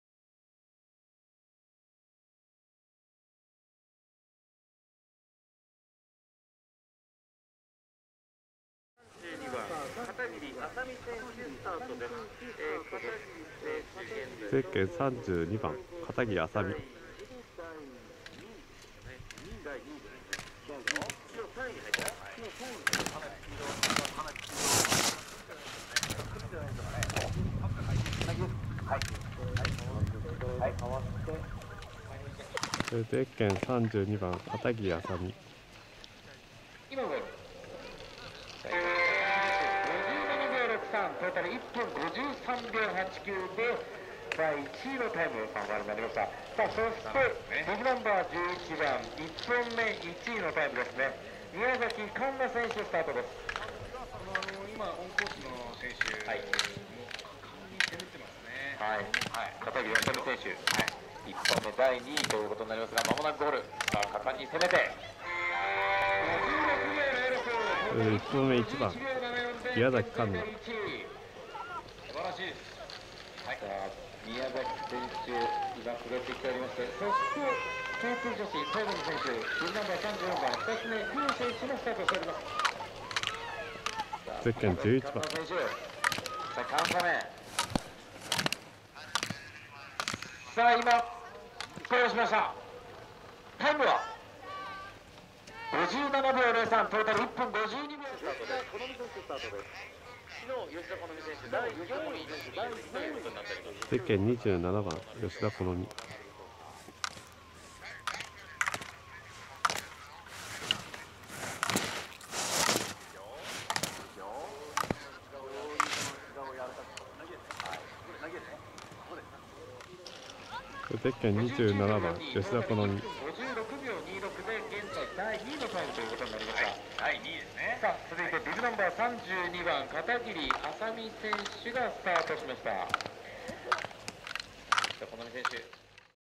第６回北海道チルドレン選手権 Ｋ２女子 WESTBERG CUP上位３名
平成２６年２月１５・１６日の２日間、朝里川温泉スキー場で開催された第６回北海道チルドレン選手権 ＷＥＳＴＢＥＲＧＣＵＰの上位３名
１位　片桐麻海　、２位　木場海怜、３位　宮崎栞奈（２本目のみ）